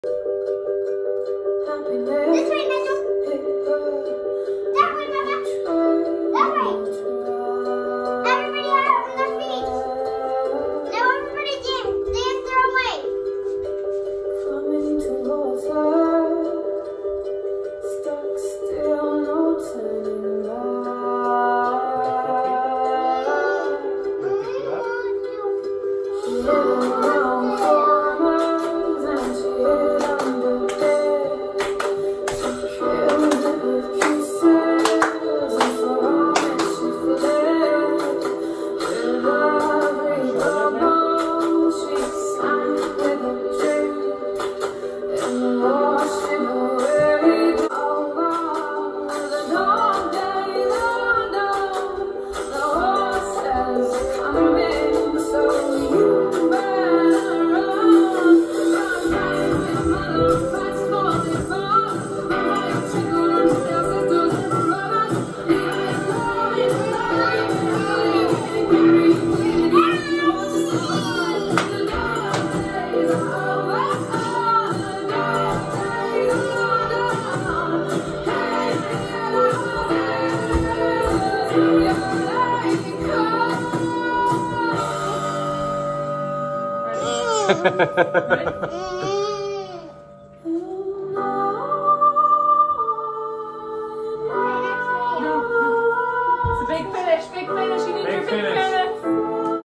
“The Dog Days of the Quarantine.” Family dance party -3-31-2020
Yesterday was beautiful and awesome. All day I thought, “ Wow! Yes! We got this!”
Today...not so much.
It was rough for me and I felt like a failure. Anxiety draining my joy.

BUT THEN, the girl who has been depleting my energy ALL DAY ordered us to have a dance break tonight 10 min before their bedtime. Through her bossy choreography we had the best 3 minutes (before she changed the song without that big finish and told us to get off her stage). 😬😂😳

The dog days certainly aren't over yet...but it sure feels better dancing through them with the people you love! 🙌💪🥰